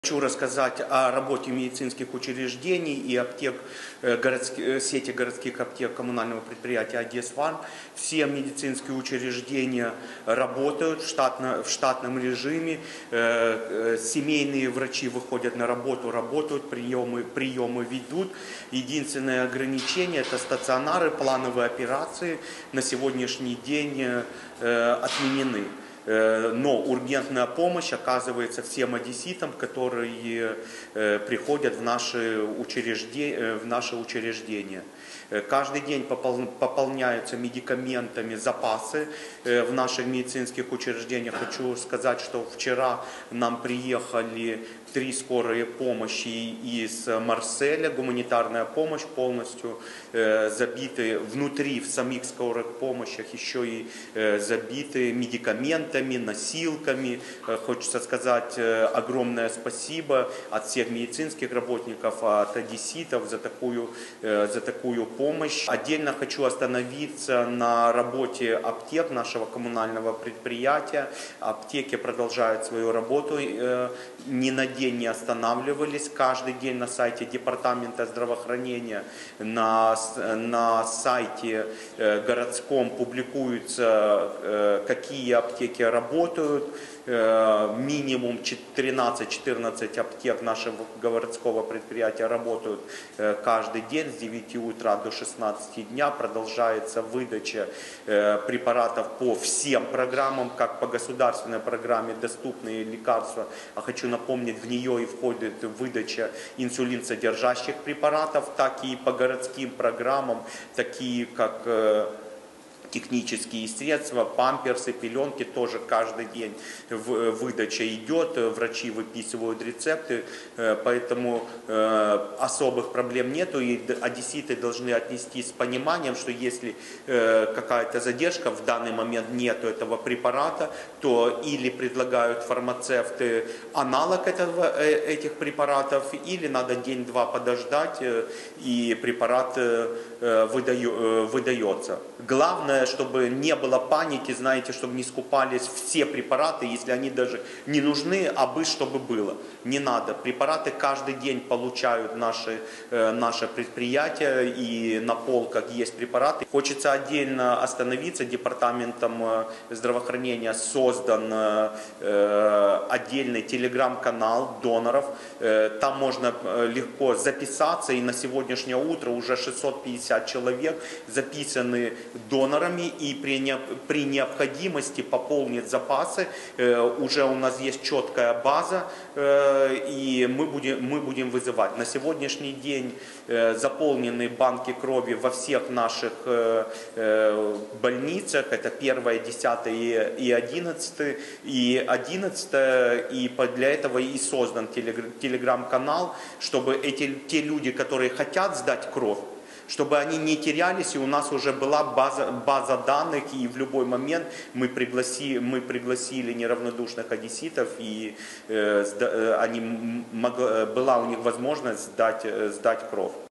Усі медустанови Одеси продовжують працювати
Левон Нікогосян, директор міського департаменту охорони здоров'я:

"Усі медустанови Одеси продовжують працювати. Сімейні лікарі ведуть прийом. У лікарнях проводяться ургентні операції, планові операції відмінено.
Щодня запаси ліків поповнюються.
Міські аптеки "Одесфарм" продовжують працювати та забезпечувати одеситів ліками, у тому числі інсулінами та препаратами за програмою "Доступні ліки". Постачання ліків отримуємо щодня, прохання до одеситів не піддаватися паніці та не скуповувати великі запаси непотрібних препаратів.
Також ми сьогодні запустили телеграм-канал із пошуку донорів крові, вже записалися 650 людей”.